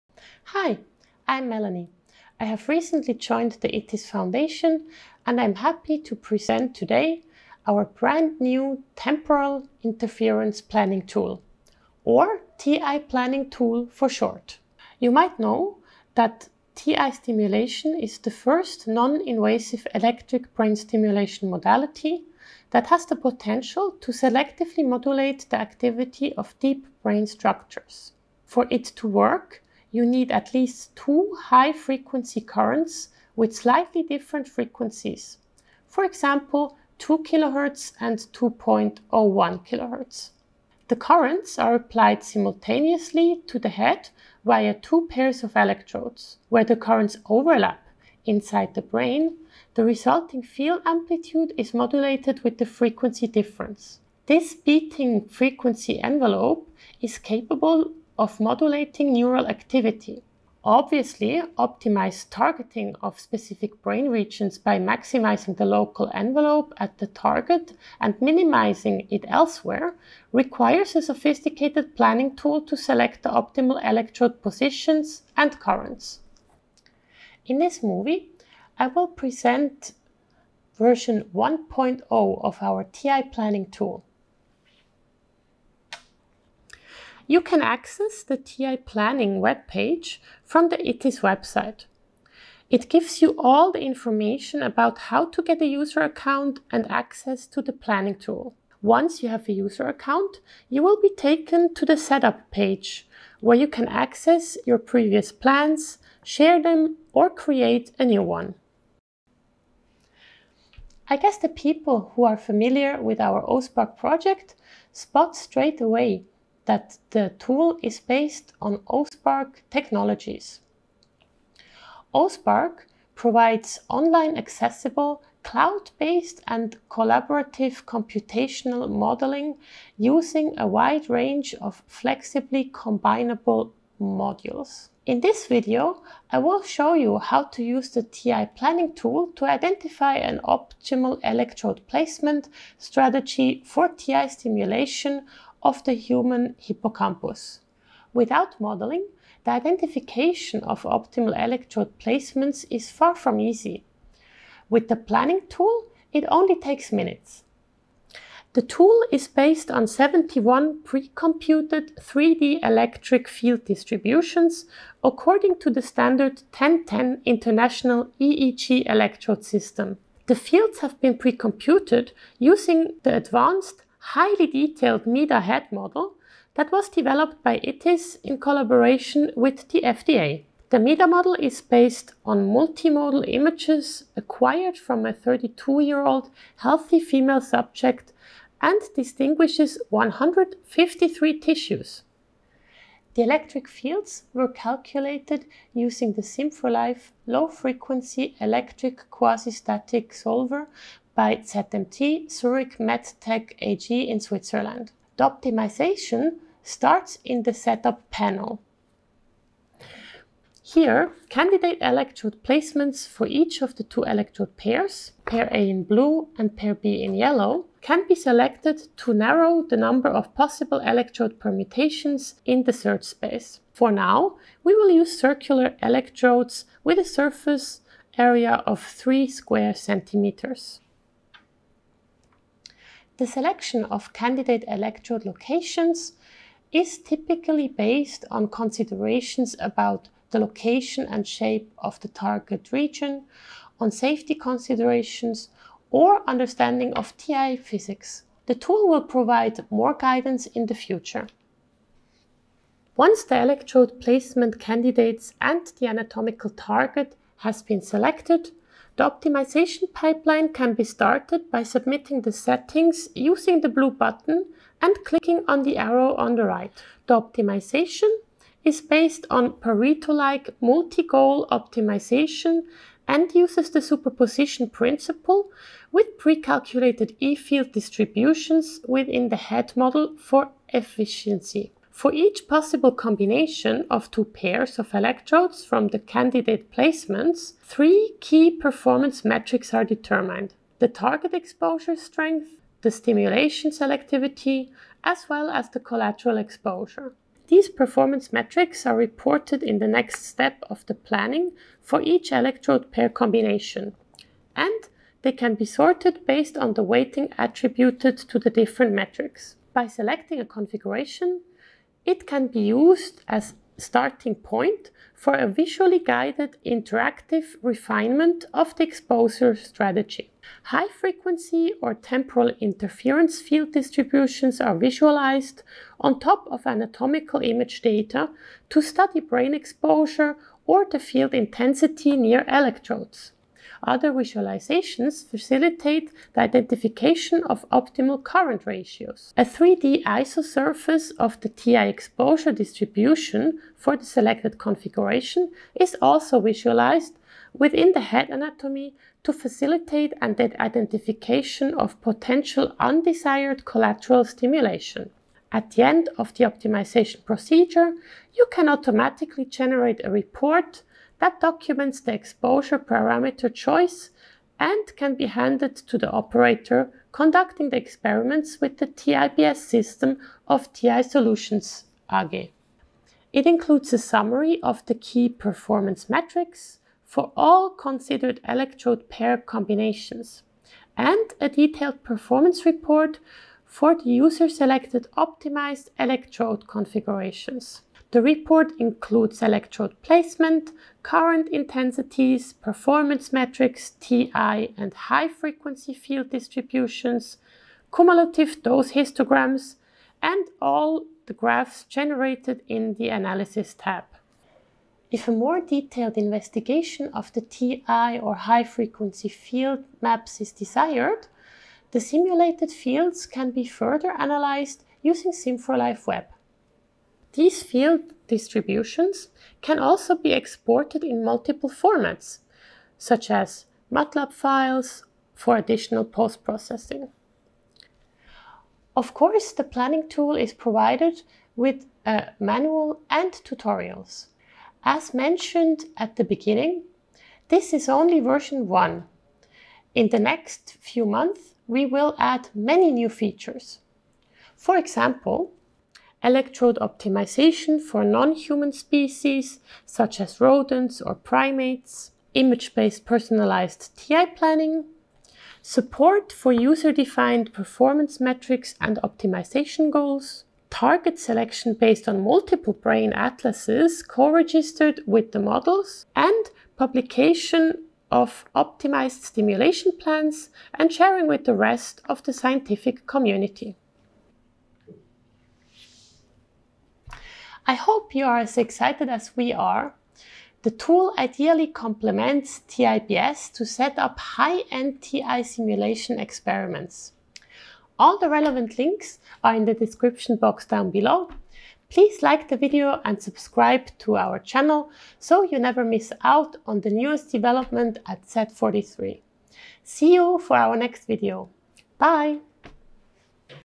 Release of Temporal Interference Planning Tool V1.0
Temporal interference stimulation (TI) is the first noninvasive brain stimulation modality capable of selectively modulating the activity of deep brain structures. To achieve optimal targeting of specific brain regions, IT'IS has released an online TI planning tool (TIP V1.0) for selecting optimal electrode positions and currents. The video shows how to access TIP V1.0 easily from the website and how it can be used to determine optimal electrode placement for TI stimulation of the human left hippocampus based on two pairs of electrodes.